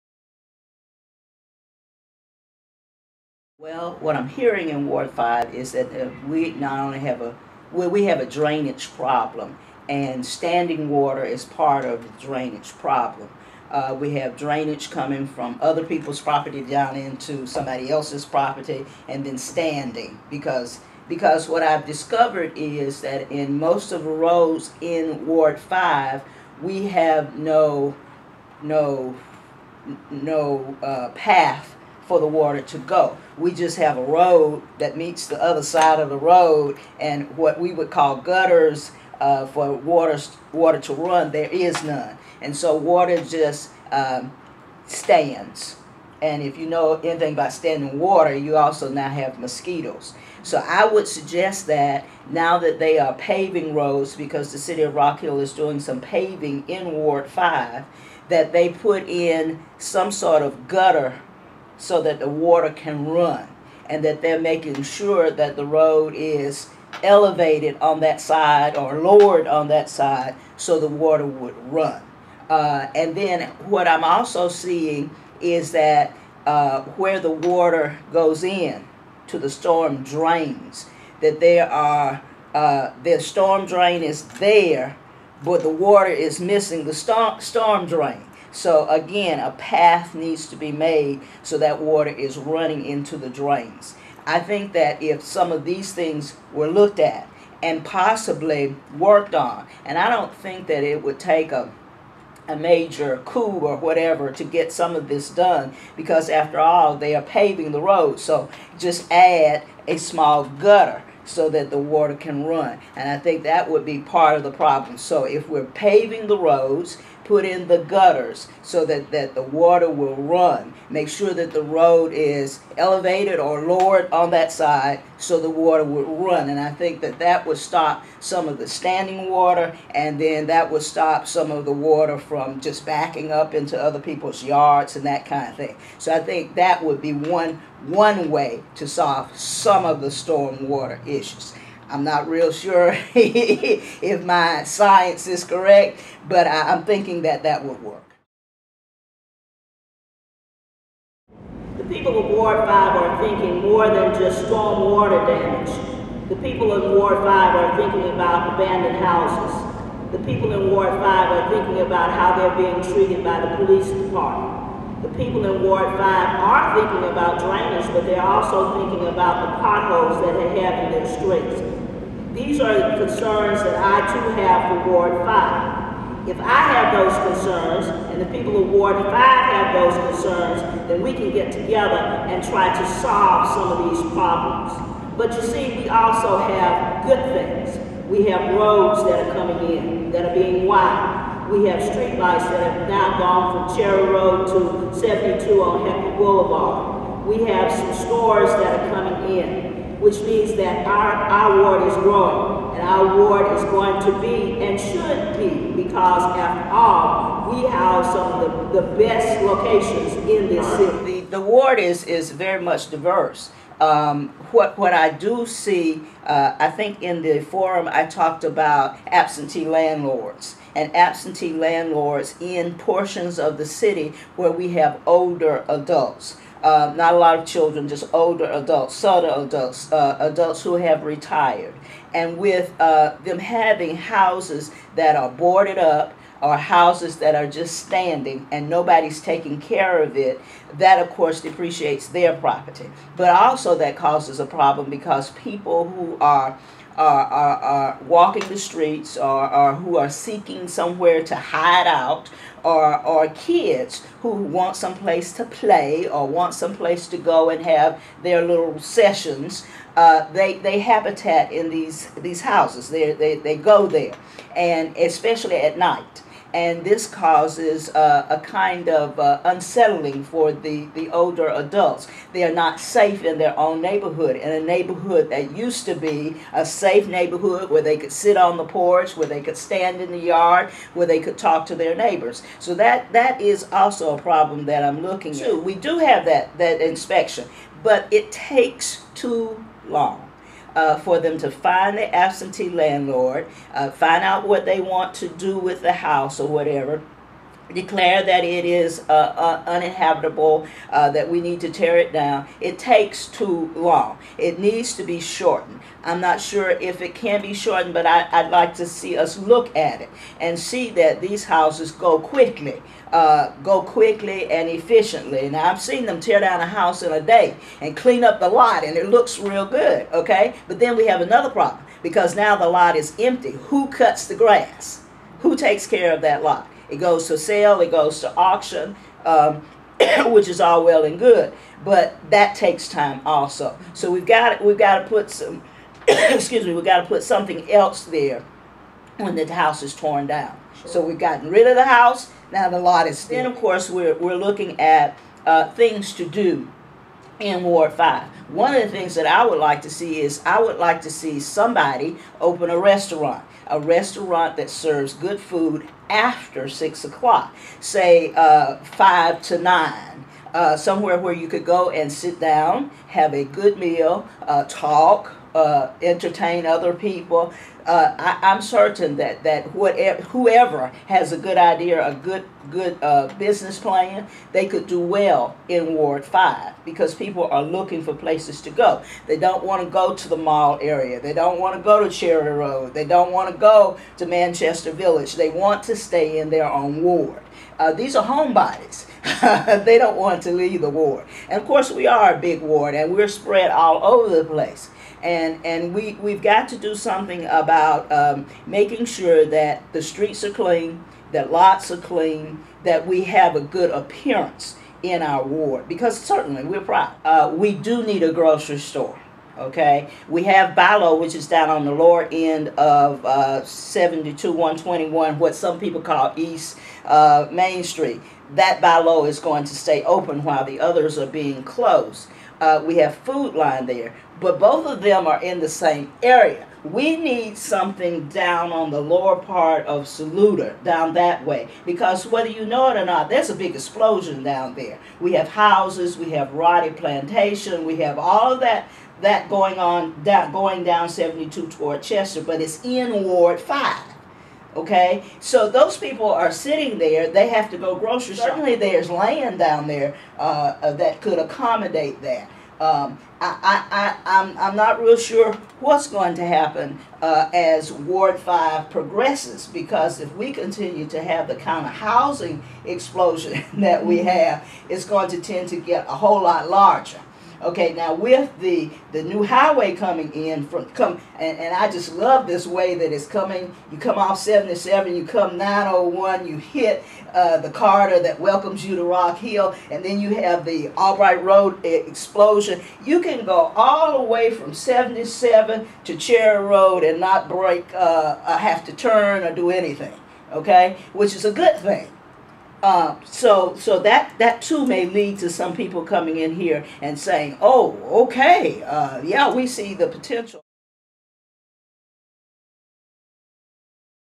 Mildred A. Moore, Ward 5 candidate for Rock Hill
All three candidates running for the Ward 5 seat on Rock Hill's City Council sat down for individual interviews with The Herald in September. The election will be held Oct. 15. The Ward 5 seat is the only contested race in Rock Hill's election. Candidates Nikita Jackson, Mildred A. Moore and Ann Williamson-Morrison are vying for the seat.